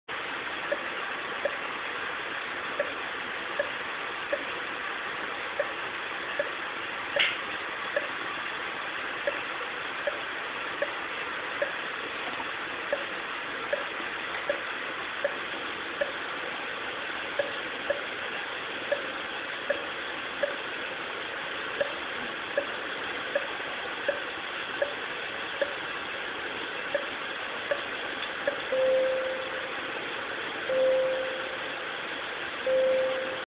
ECG Video: AV Block - Second Degree Type 1 (Wenckebach) - video2
A relatively benign AV block, which does not usually progress to complete AV Block.